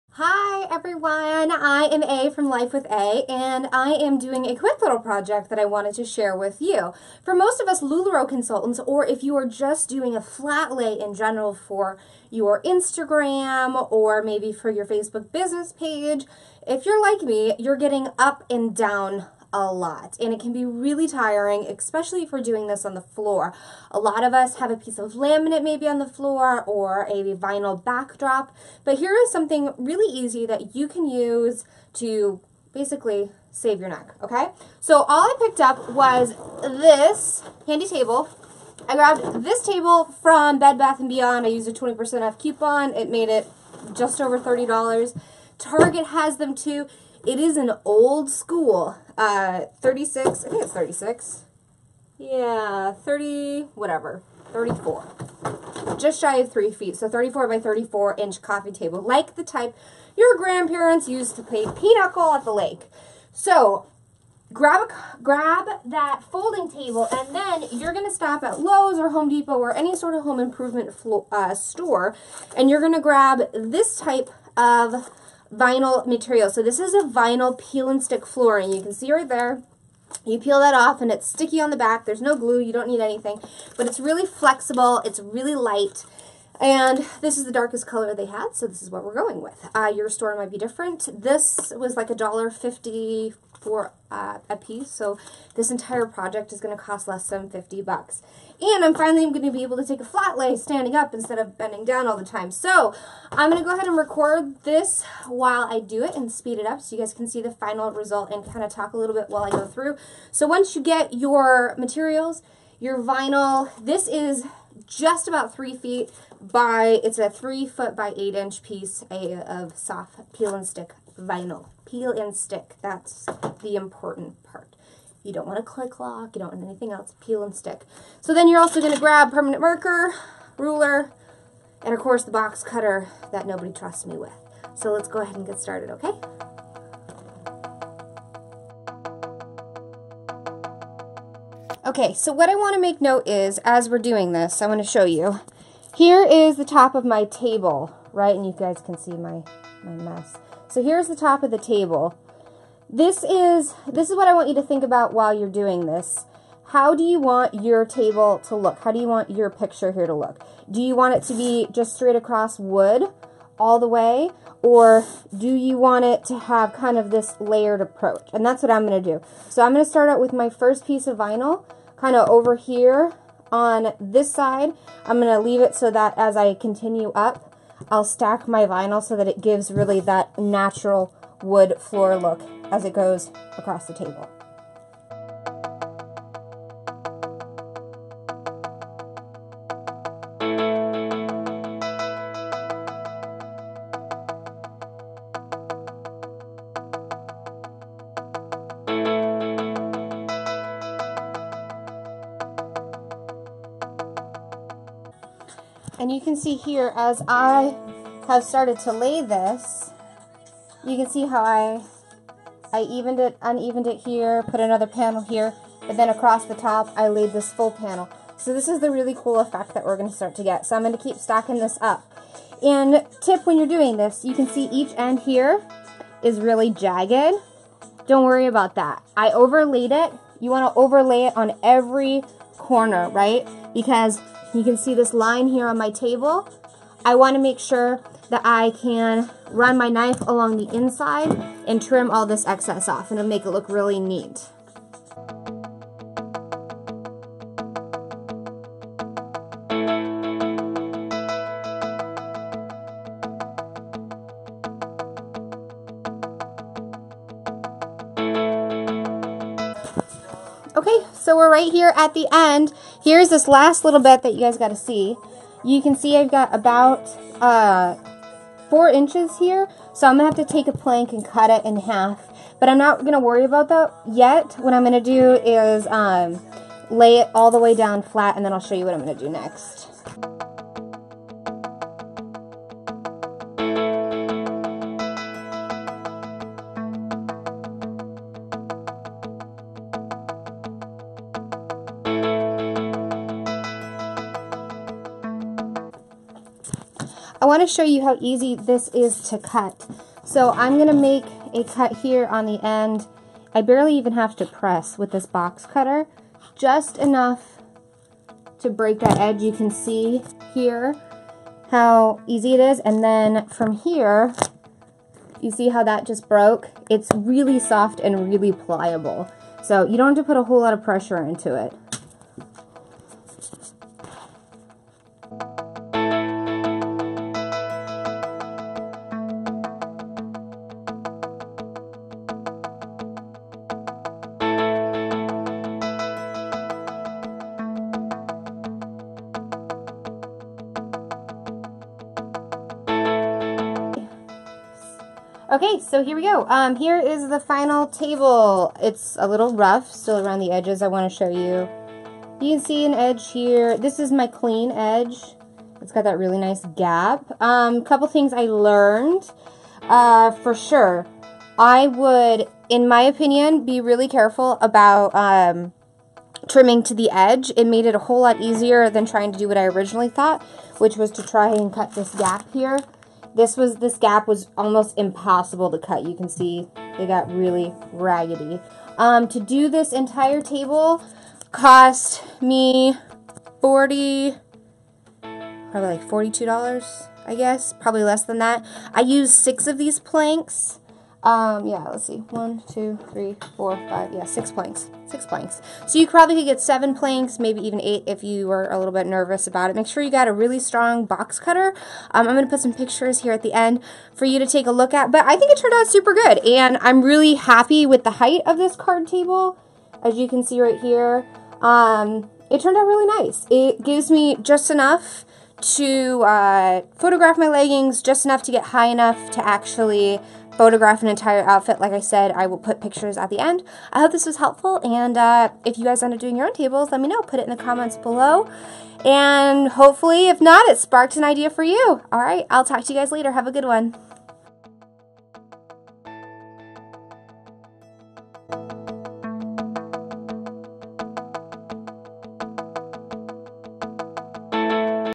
Create Your Own Flat Lay Table
How to make your own Table for Flat Flay Photographs.

I was EXHAUSTED. yes really that much, in the process of my current flat lay strategy.

So I went looking for a low cost solution & I came up with something that is working amazing! I

Materials:
1 Card Table. You can find them at Bed Bath & Beyond, Target or the good old Amazon

2. Peel & Stick Laminate. I got mine at Lowes. You can find lots of options on guess where...Amazon. For a quick fix check your Home Depot or Lowes.

3. Box Cutter --SHARP--

4. About 30-45 minutes.